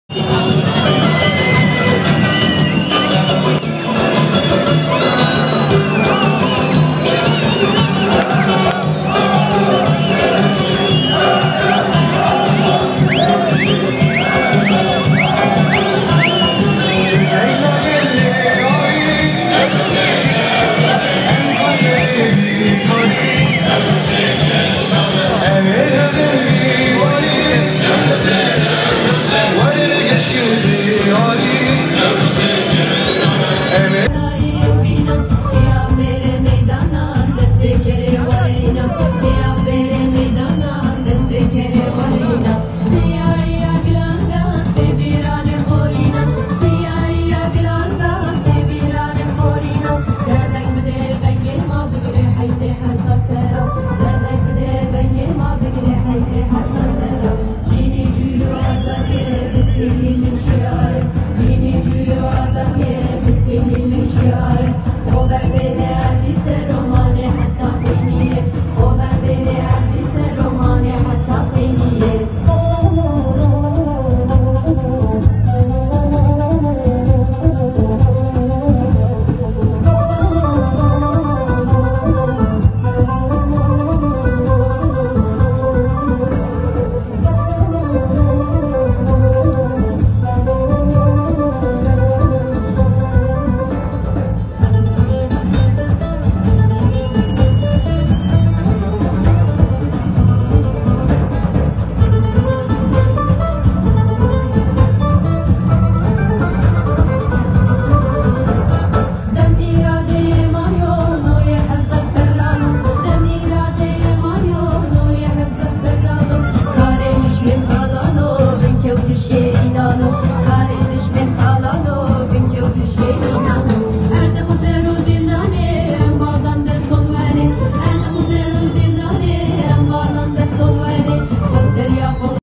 İzmir'de Newroz büyük bir coşkuyla başladı
İzmir’de, bu yıl "Rabe dema azadî û serkeftinê ye" şiarıyla gerçekleştirilen Newroz kutlaması, Gündoğdu Meydanı'nda binlerce kişinin katılımıyla başladı.